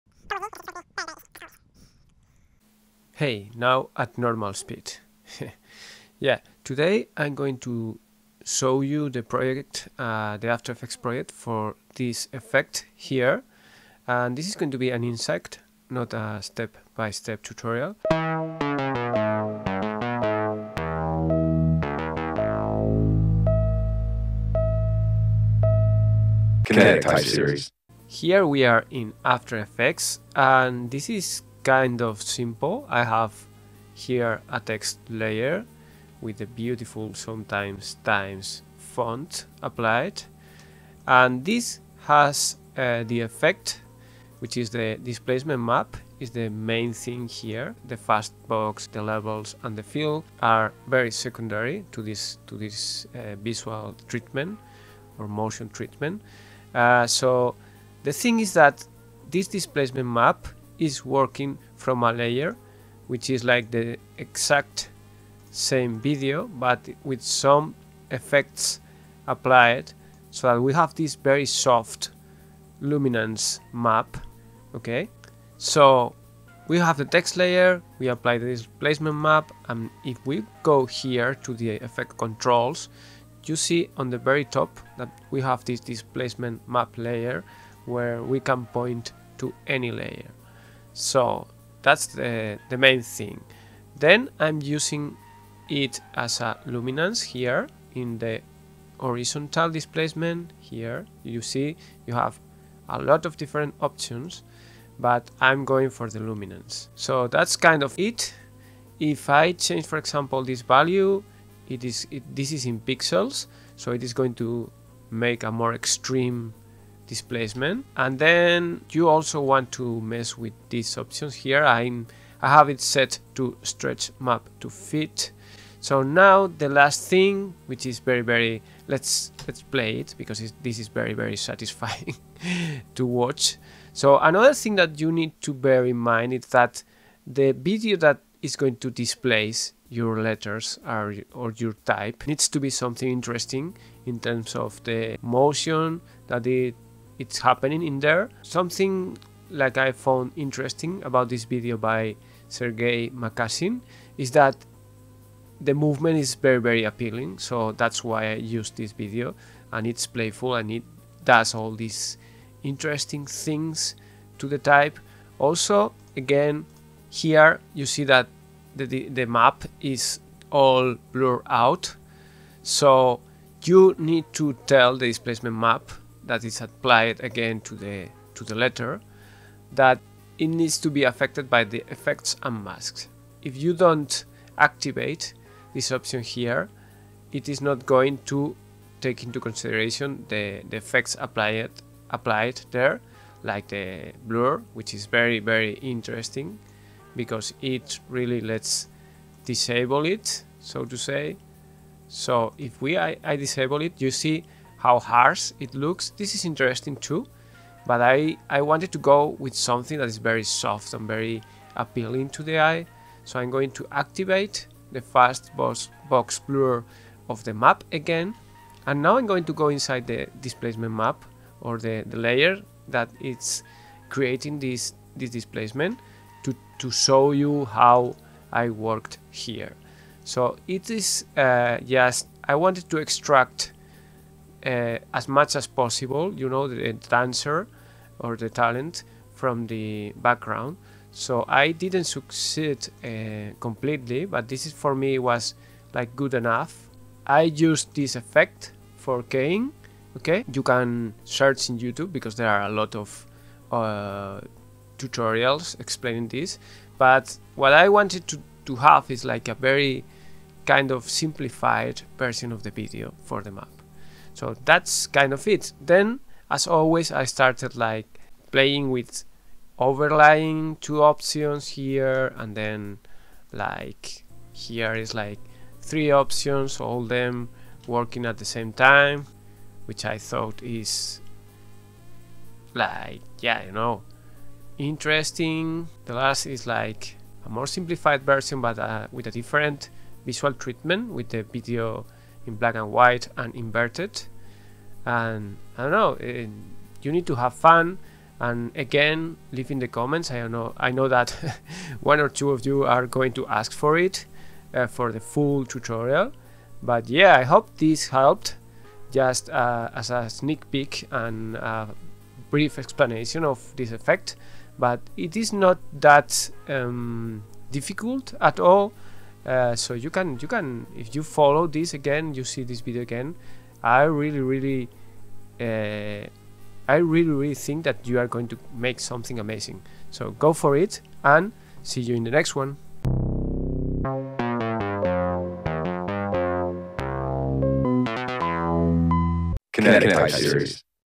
KTS® - ABC% Displacement - After Effects
KTS® - ABC% Displacement in After Effects
I do the channel for free and in my spare time. If you want to consider a contribution, I will appreciate it a lot:
Effects used:
Displacement Map
Difference Matte
Soundtrack is:
by Monroeville Music Center .